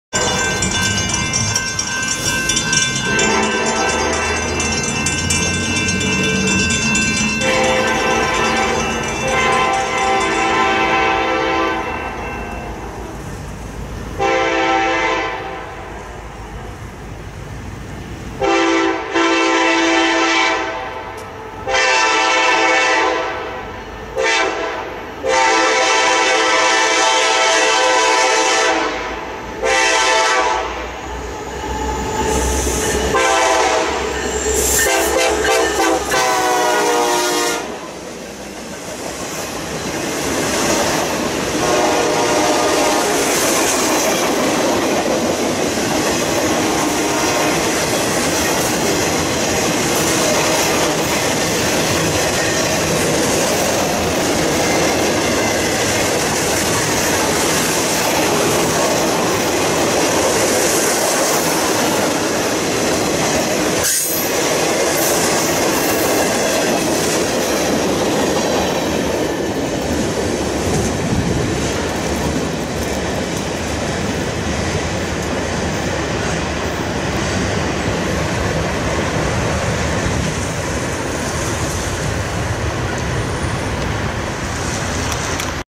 BNSF 8503 {SB} Leads CN Rail Train - Brookhaven, MS 12.15.18
Wasn't expecting this baby! Came out of no where. Out in the rain waiting for Amtrak 59. This BNSF lead rail train had a friendly crew with horn salute.

Train 1 - BNSF 8503
Direction - South Bound
Speed  - 40 mph
City - Brookhaven, MS'
MP - 783
CN Gulf Division Mc Comb Sub